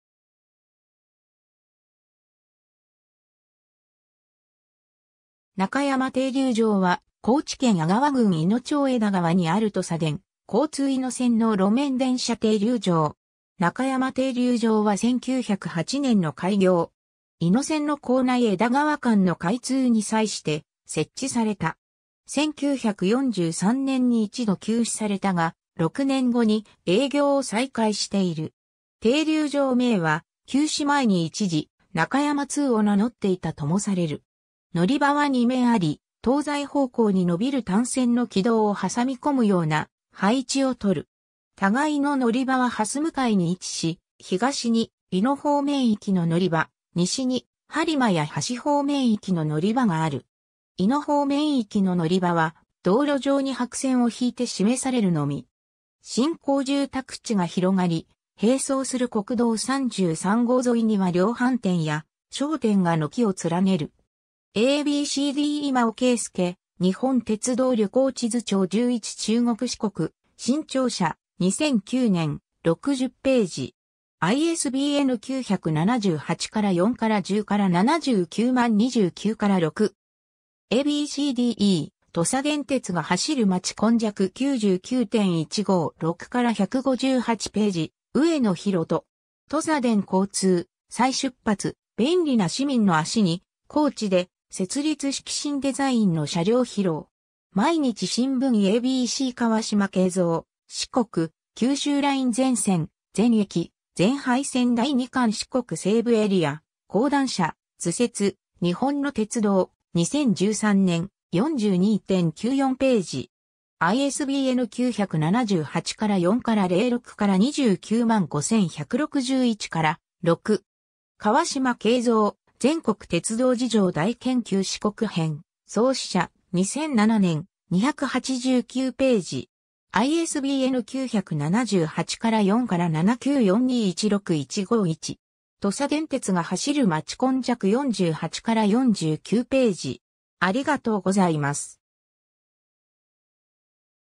中山停留場
中山停留場（なかやまていりゅうじょう）は、高知県吾川郡いの町枝川にあるとさでん交通伊野線の路面電車停留場。
 中山停留場は1908年（明治41年）の開業。
伊野線の咥内 - 枝川間の開通に際して設置された。
1943年（昭和18年）に一度休止されたが、6年後に営業を再開している。
 停留場名は休止前に一時「中山通」を名乗っていたともされる。
 乗り場は2面あり、東西方向に伸びる単線の軌道を挟み込むような配置をとる。
互いの乗り場は斜向かいに位置し、東に伊野方面行きの乗り場、西にはりまや橋方面行きの乗り場がある。
伊野方面行きの乗り場は道路上に白線を引いて示されるのみ。
 新興住宅地が広がり、並走する国道33号沿いには量販店や商店が軒を連ねる。
 ^ a b c d 今尾恵介（監修）『日本鉄道旅行地図帳』11 中国四国、新潮社、2009年、60頁。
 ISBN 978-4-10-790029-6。
 ^ a b c d e 『土佐電鉄が走る街 今昔』99・156-158頁 ^ 上野宏人 (2014年10月2日). “とさでん交通：「再出発」 「便利な市民の足に」高知で設立式 新デザインの車両披露”. 毎日新聞 (毎日新聞社) ^ a b c 川島令三『四国・九州ライン 全線・全駅・全配線』第2巻 四国西部エリア、講談社〈【図説】 日本の鉄道〉、2013年、42・94頁。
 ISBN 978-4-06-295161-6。
 ^ 川島令三『全国鉄道事情大研究』四国篇、草思社、2007年、289頁。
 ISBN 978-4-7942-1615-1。
 ^ 『土佐電鉄が走る街 今昔』48-49頁